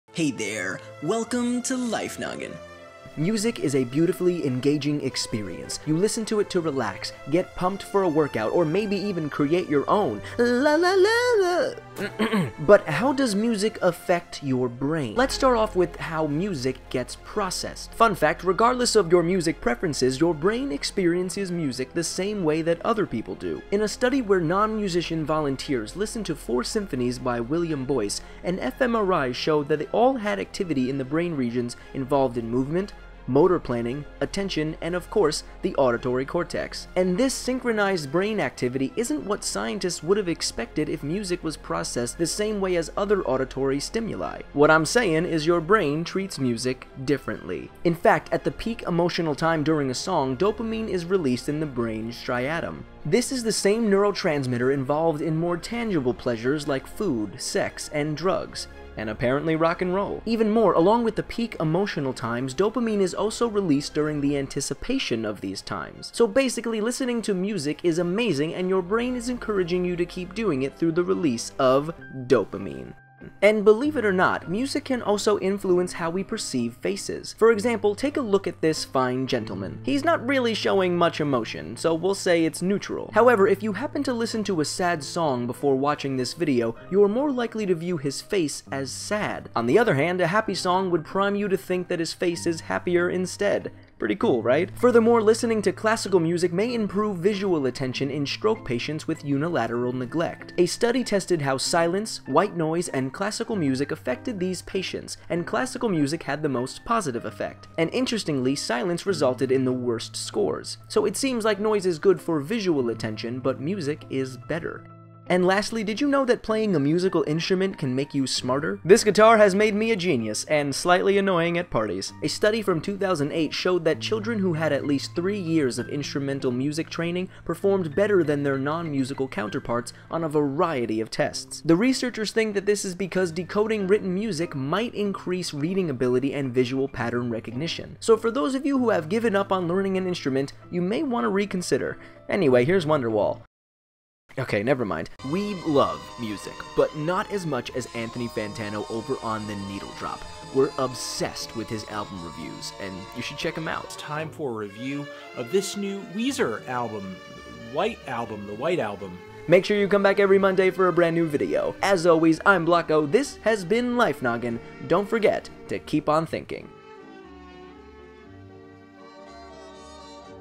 This Is Your Brain On Music
What happens to your brain when you listen to music?